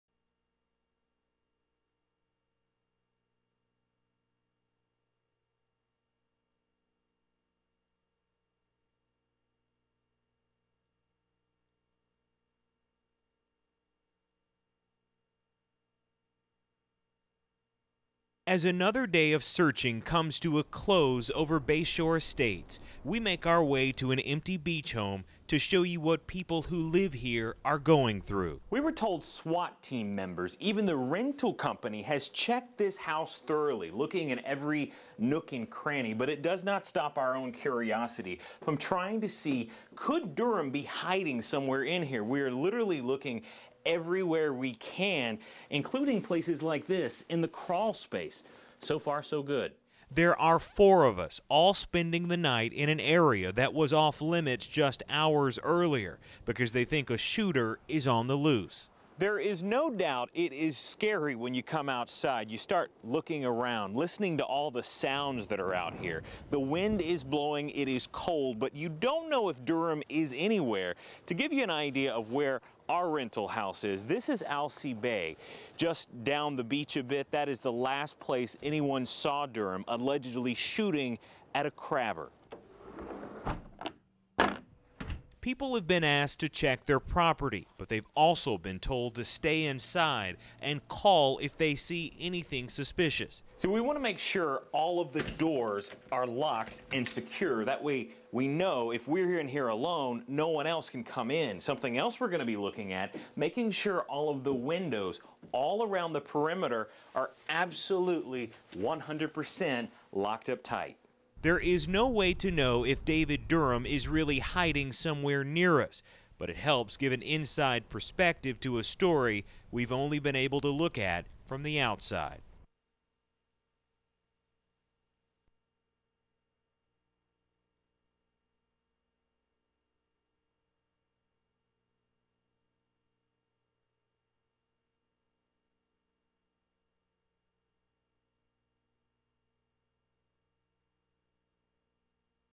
Waldport Fugitive
The 7th or so story after a 5 day manhunt on the Oregon coast. We devised a plan to stay in the rental house area where the suspect was supposedly hiding giving the viewer a first hand look at what things have been like for residence.